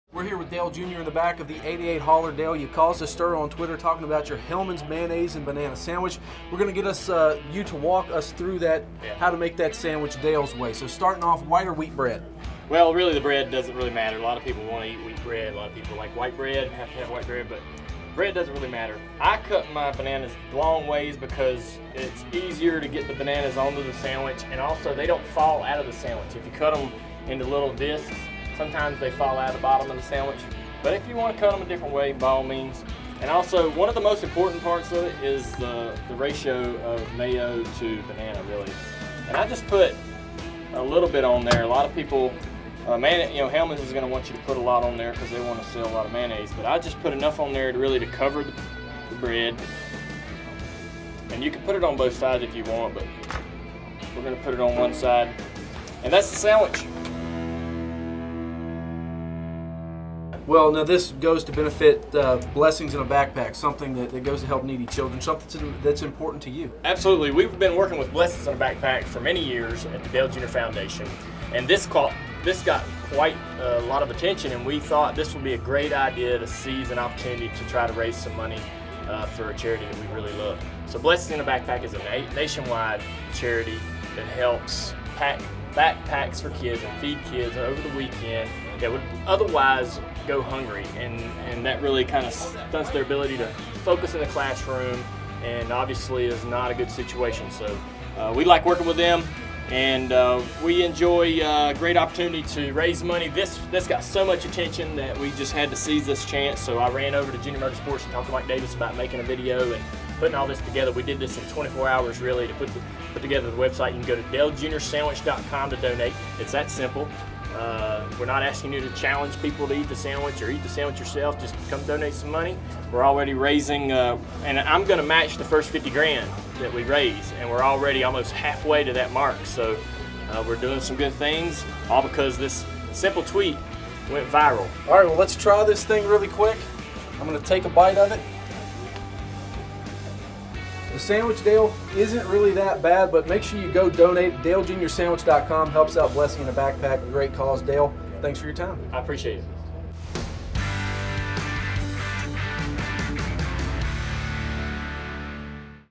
Food for thought: How to make the perfect banana-mayo sandwich
NASCAR.com’s Jonathan Merryman chats with Dale Earnhardt Jr. about his Hellmann’s banana-mayo sandwich, as well as, how Dale Jr. has turned the publicity into the Dale Jr. Sandwich fundraiser where all proceeds will go to Blessings in a Backpack.